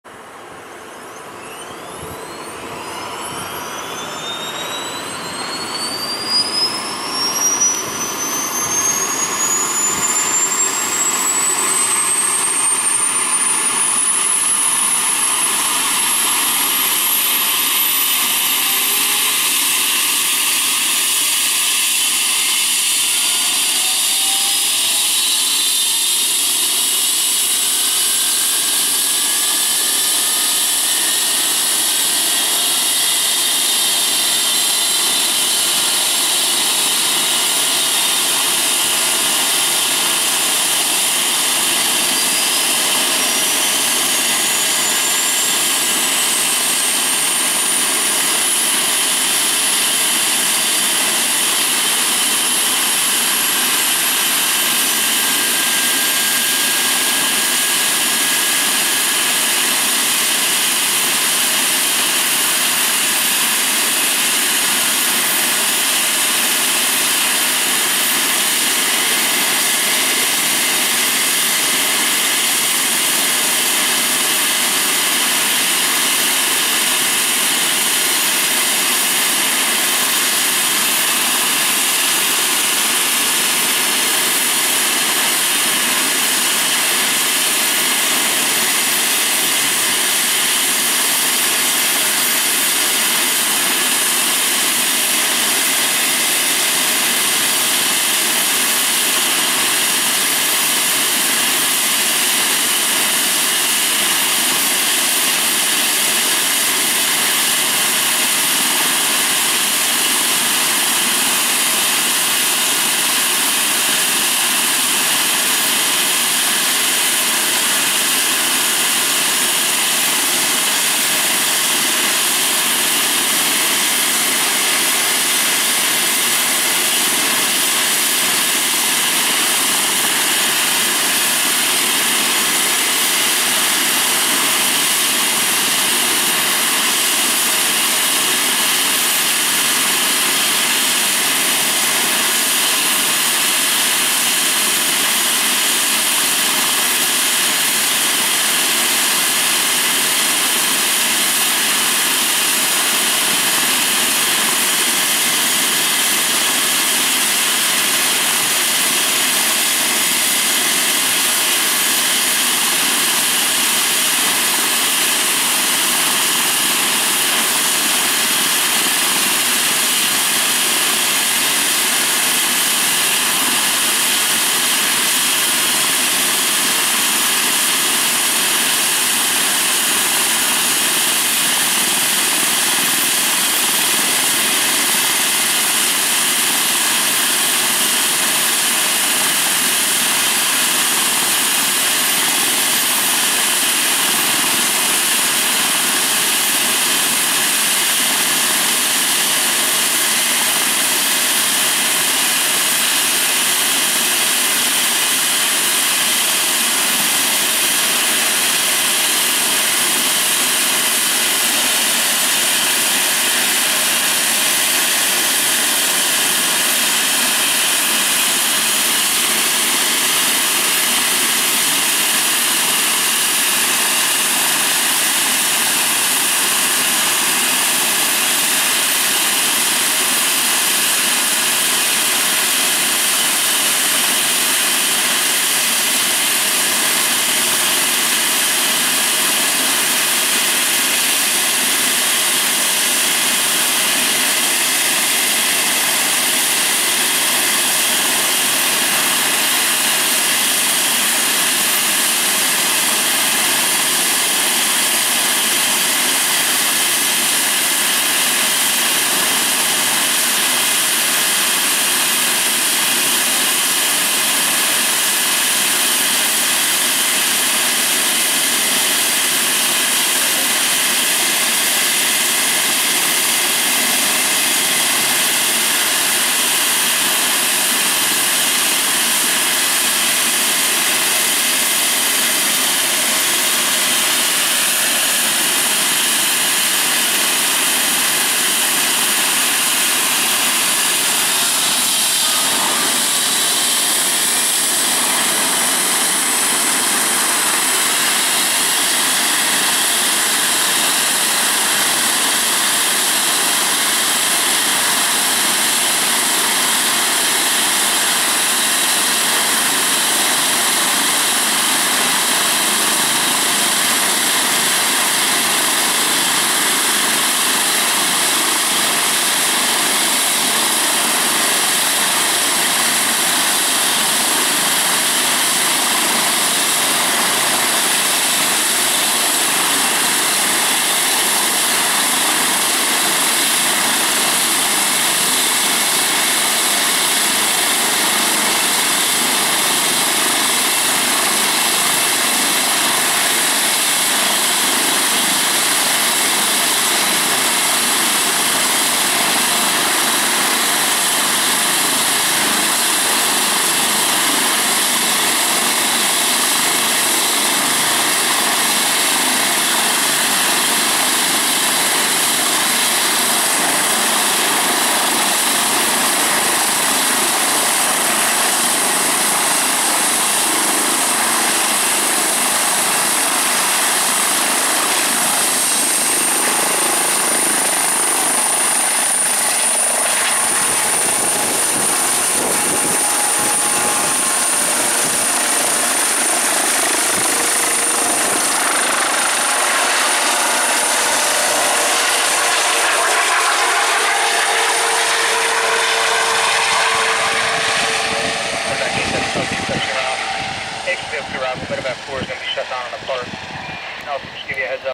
STAT MedEvac 8 N944ME Taking Off From Allegheny County Airport
2010 EC135T2+/N944ME/STAT MedEvac